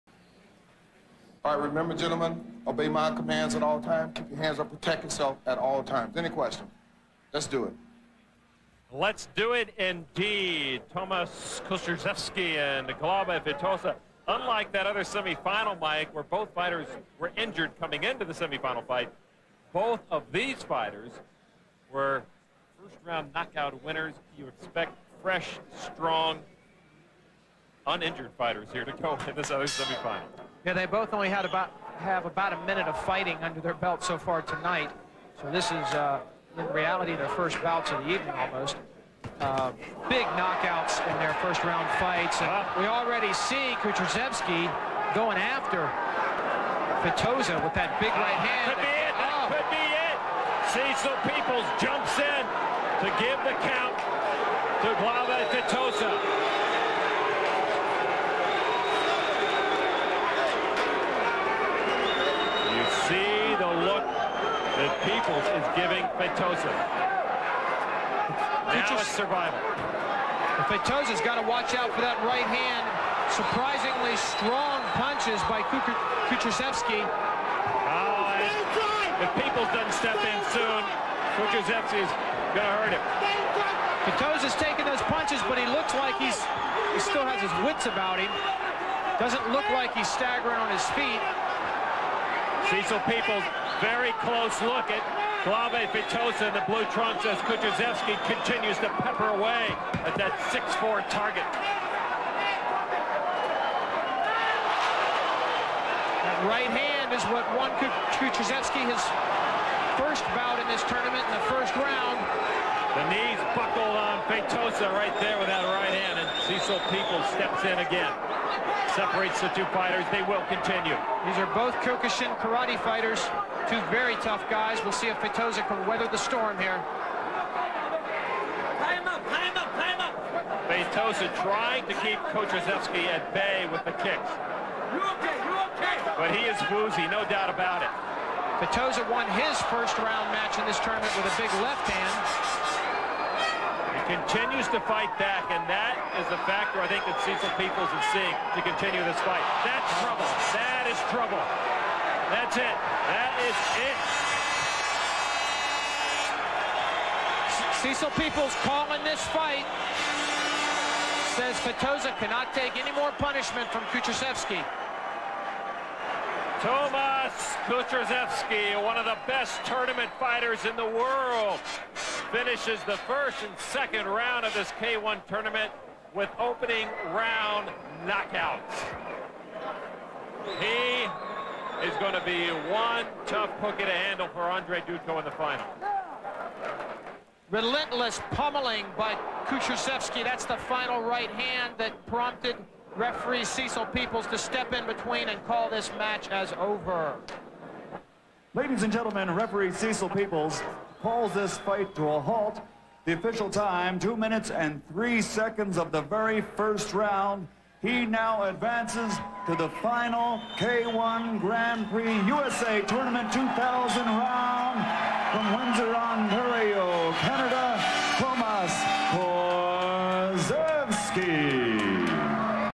Tomasz Kucharzewski Vs. Glaube Feitosa (05/08/2000)
Tomasz Kucharzewski Vs. Glaube Feitosa
K-1 USA Championships 2000
August 5, 2000 | Las Vegas, USA

Heavyweight bout with K-1 rules, and tournament semifinal.